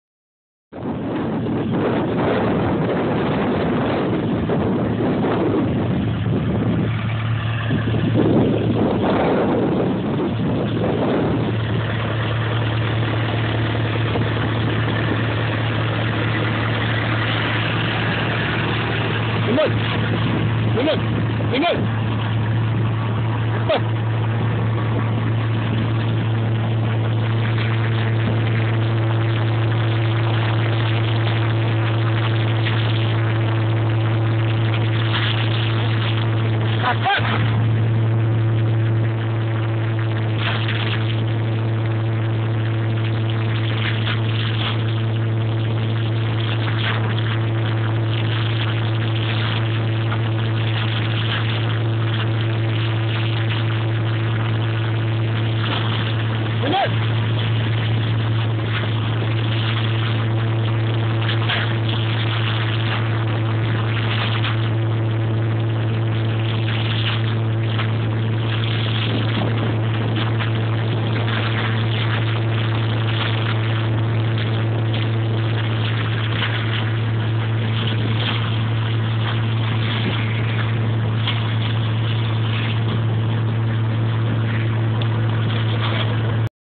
0814 1ere teste BROUILLEUR F
c'est un teste qui est fait après avoir enlever la gaine de goutte à goutte.
la qualité de coupe est bonne,
voir influence de la roue de niveau sur les racines. 0814 1ere teste BROUILLEUR G